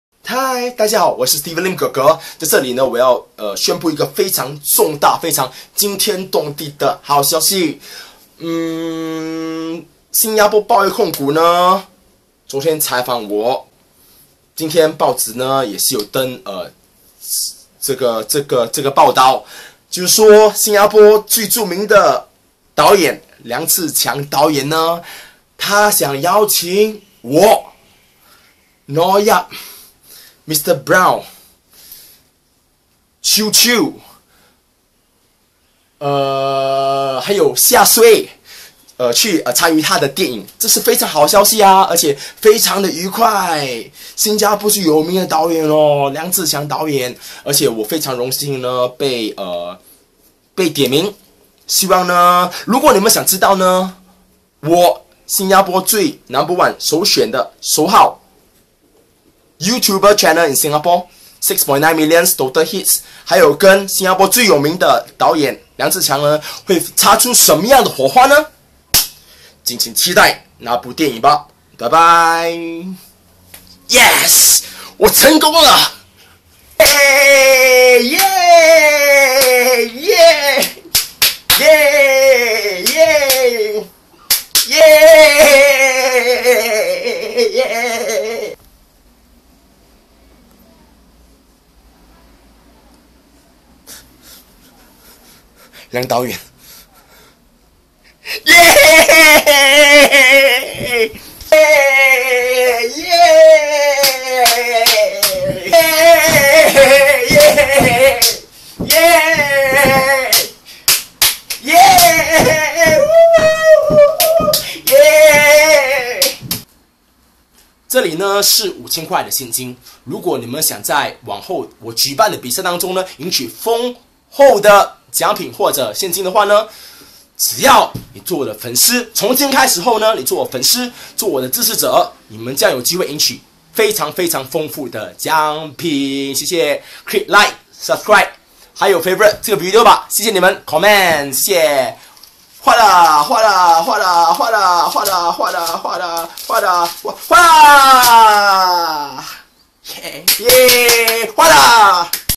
Annoucement: Famous Director Jack Neo Invites SuperStar Steven Lim to Act in His Big Movie! Share!
Steven Lim Official Annoucement: Famous Director Jack Neo Invites Singapore SuperStar Steven Lim thru SPH The New Paper article to his upcoming Movies!!! Other names mentioned are Xiaxue, Noah Yap, Mr Brown, Qiu Qiu, Peggy Heng, and Dawn Yang! SPH the New Paper and their casting asked me to do a video on this invitation.  Well, here it is!!!  This video is totally shot by Samsung Galaxy Note Back Cam with HD 1080P.

Steven Lim is going to be a Movie Star! Special thanks to Jack Neo and their casting! Share this to your friends!!